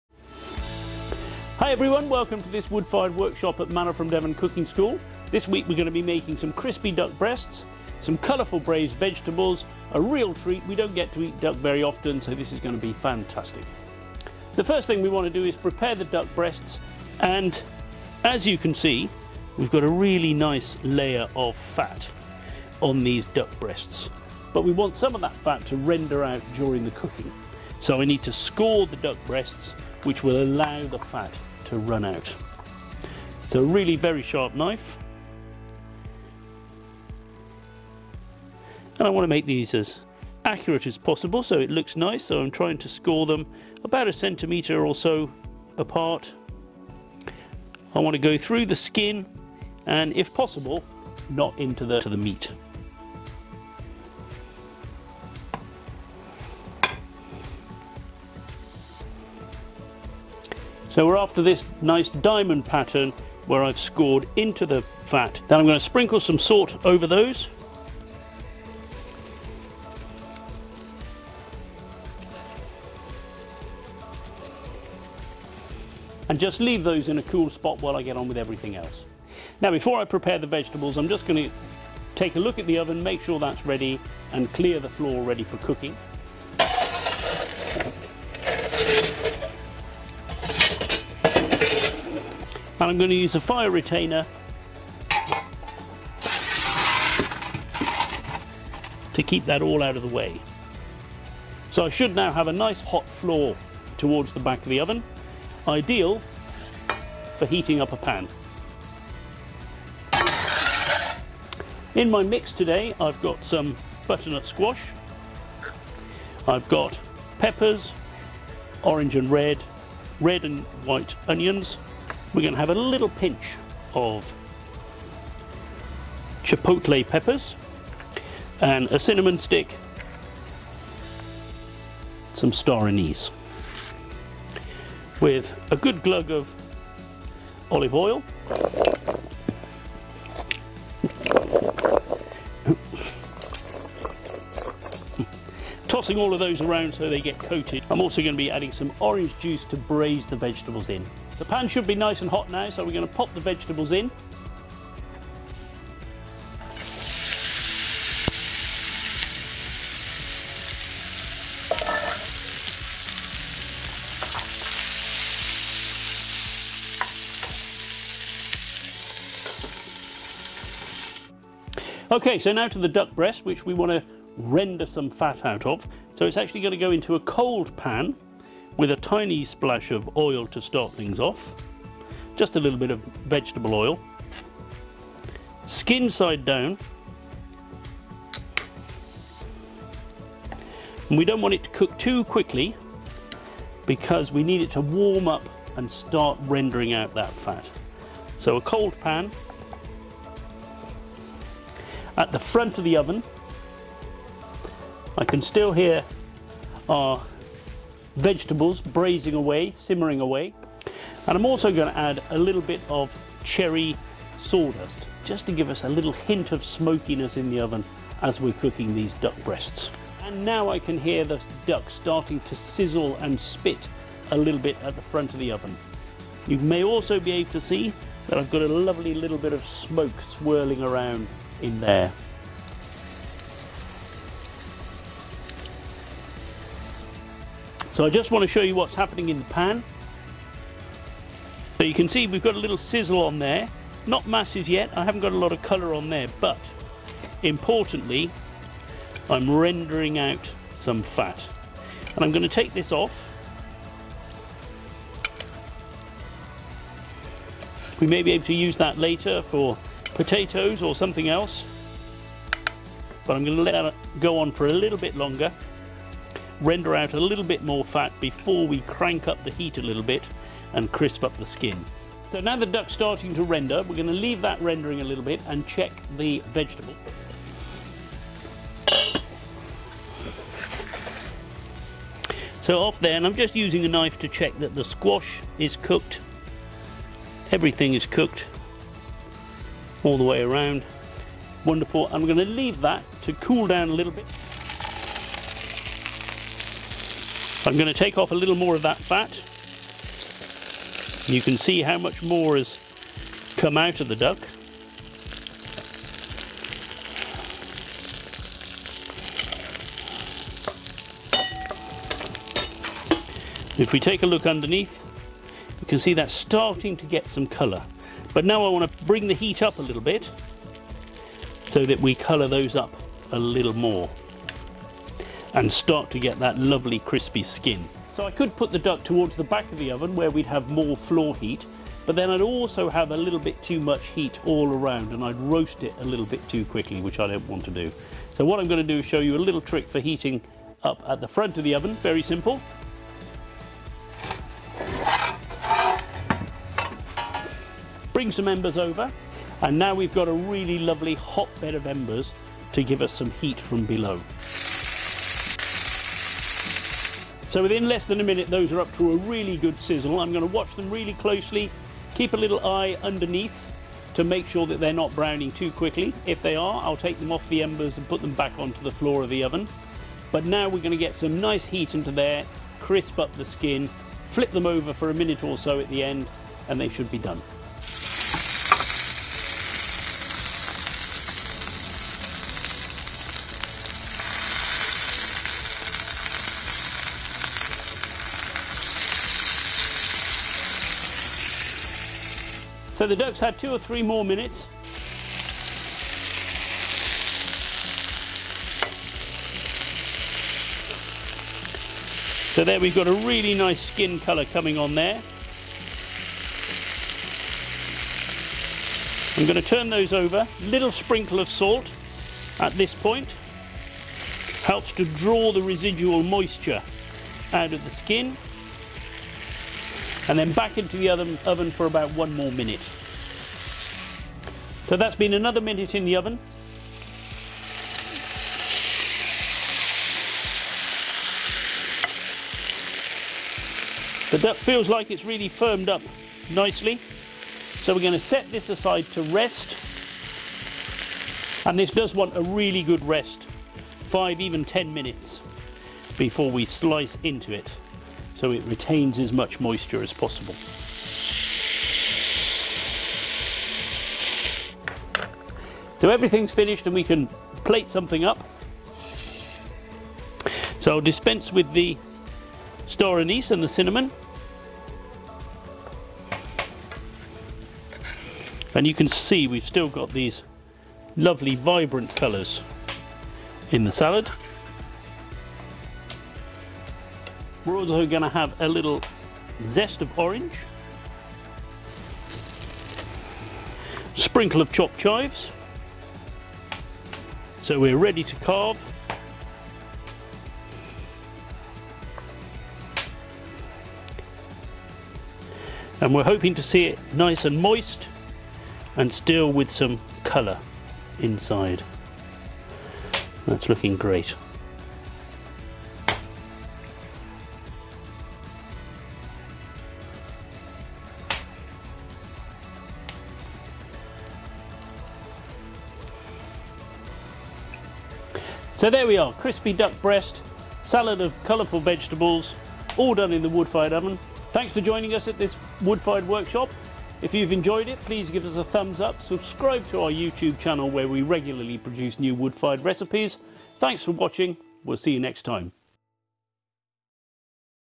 Wood Fired Duck Breast with Braised Vegetables
We love duck but we don’t eat it very often – cooking this recipe has convinced us we should eat it some more! Do try it out and let us know how you get on. We think it’s really delicious.

Succulent meat with a crispy skin and rendered fat - excellent for using when you're next cooking roast potatoes.

Happy cooking
David and Holly
Manna from Devon Woodfired Cooking School

Manna from Devon On Social Media - join us!